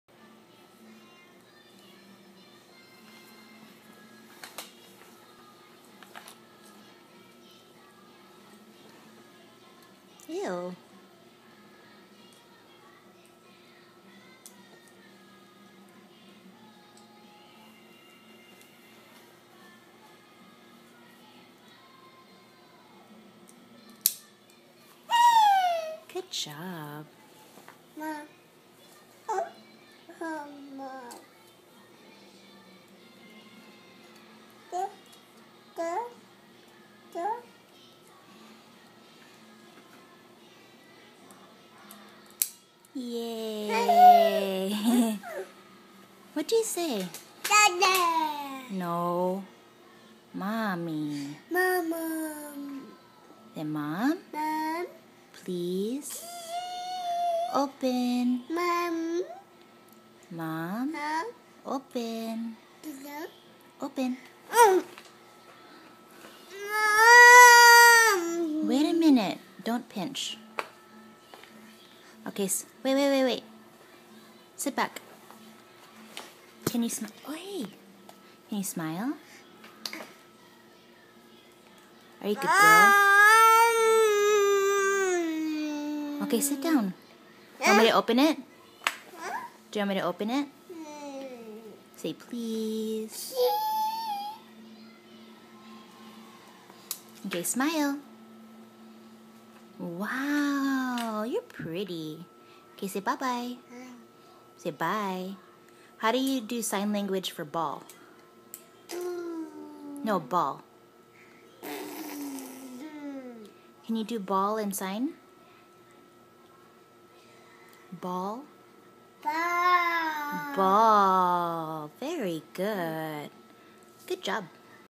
She buckles herself in
She'll buckle herself in all day but hasn't figured out how to unbuckle it so shell cry till you do it for her...crazy girl!